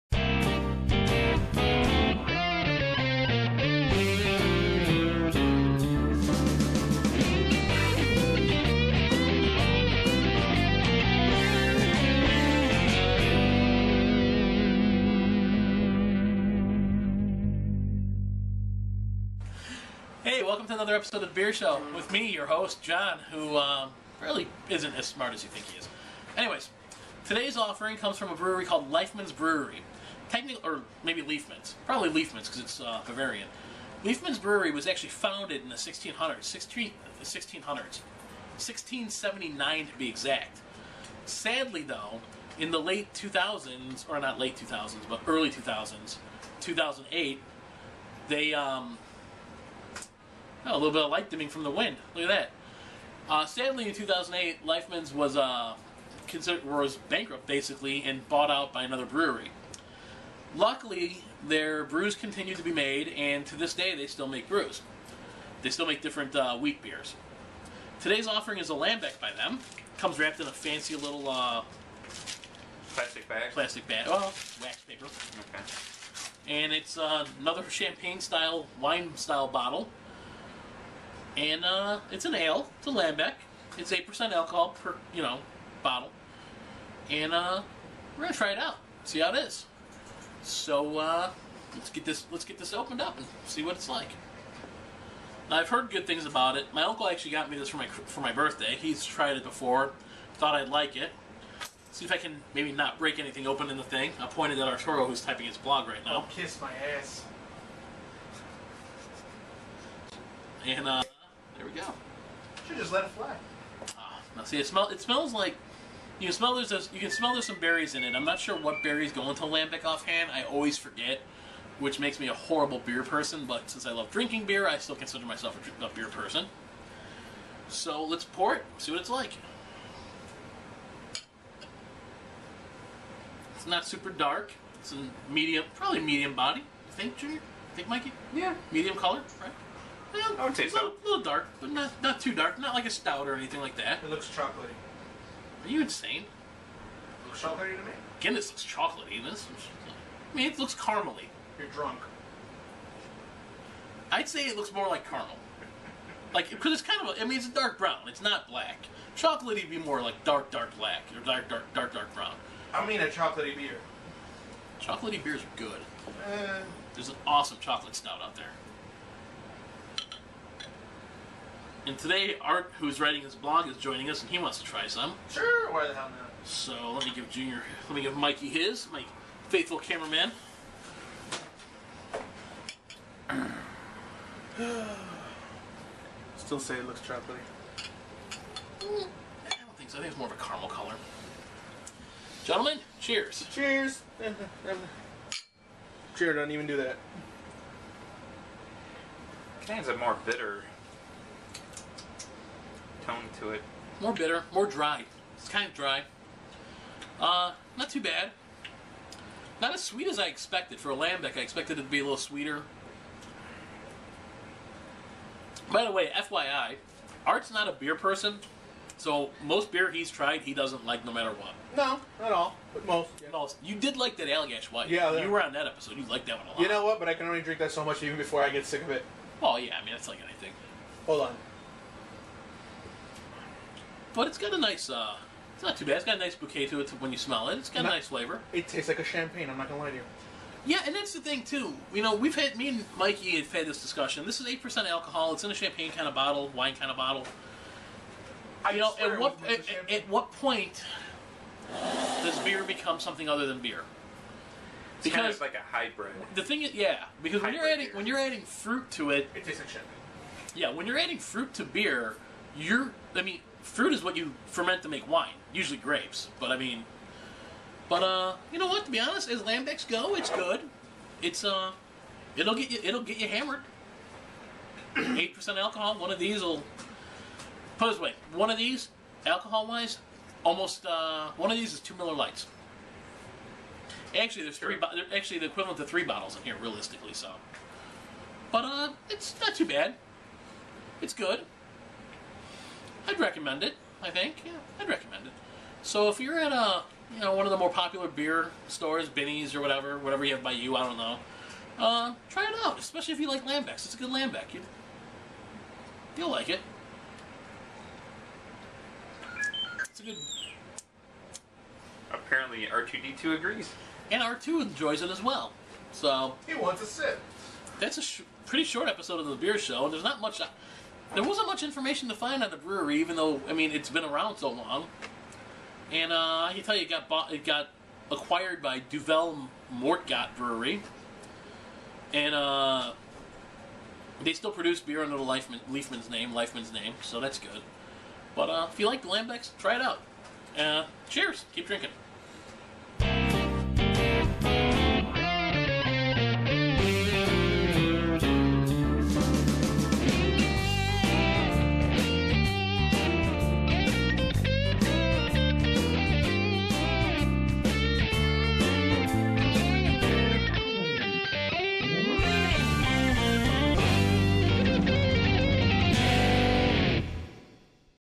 Episode 13 Liefmans Goundenband Lambic
First, sorry for the titling error, the video says episode 12, but it is actually episode 13, now that that is out of the way.

This weeks offering comes from the Belgian brewery Liefmans Oudenaarde which was founded in 1679.  While they brew a few Belgian beers, the two main ones are the Oud Bruin and the Lambic.  Sadly, the brewery went bankrupt.  It was bought by Duvel Moortgat Brewery so luckily this historic brewery and its Lambic is still available.

As for the Lambic it self?  Well that's what the video is for.  Sorry to say that I don't have the skill of a beer connoisseur, but hopefully I describe it well enough to make you want to try it out!